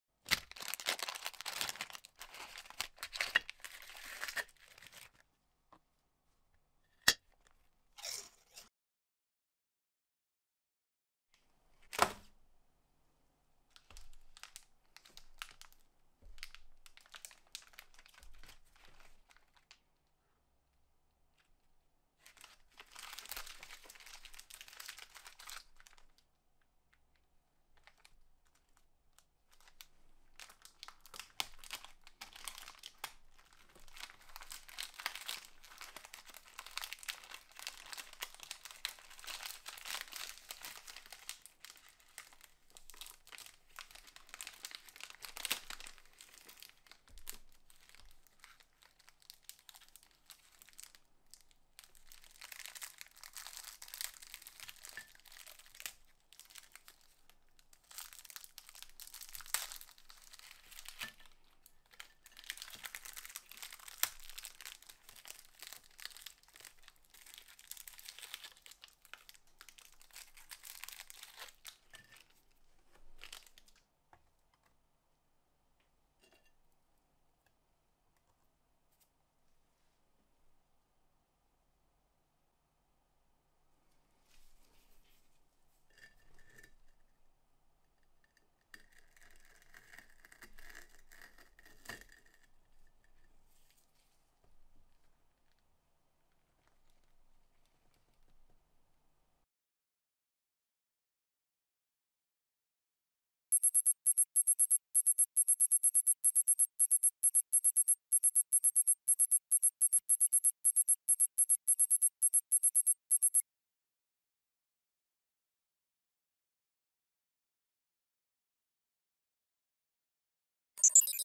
Kinder Bueno White (39g) [Judgment Day: 0020]
Snack unboxing, day 20
Today's subject: Kinder Bueno White (39g)
Verdict: 👍 (good)
I'll be snack...